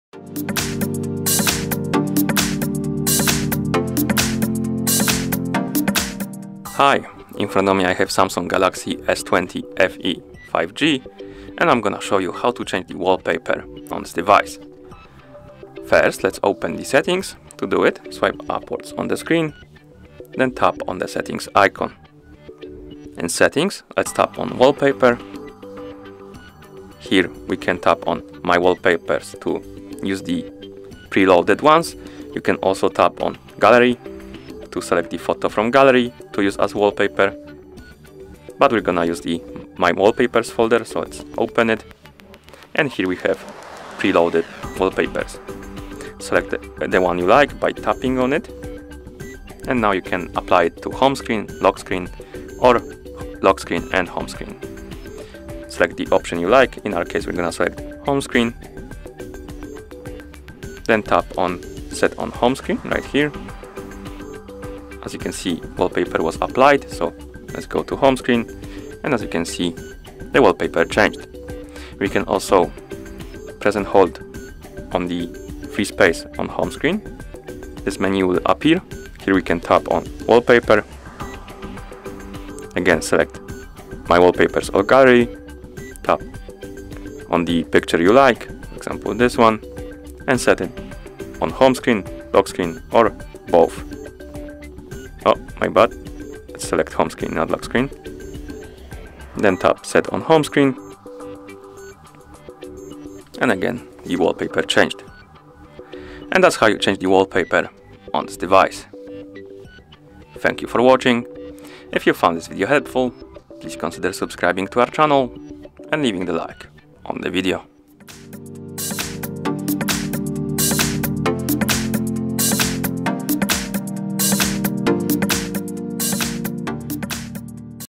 How to Change Wallpaper in SAMSUNG Galaxy S20 FE 5G – Desktop Update
Learn more info about SAMSUNG Galaxy S20 FE 5G:
If you no longer want to have the default wallpaper set on the SAMSUNG Galaxy S20 FE 5G, see where the display settings are in the SAMSUNG Galaxy S20 FE 5G. Follow our instructions and discover all available wallpapers adapted to your device. Also learn how to set a picture from the gallery as wallpaper.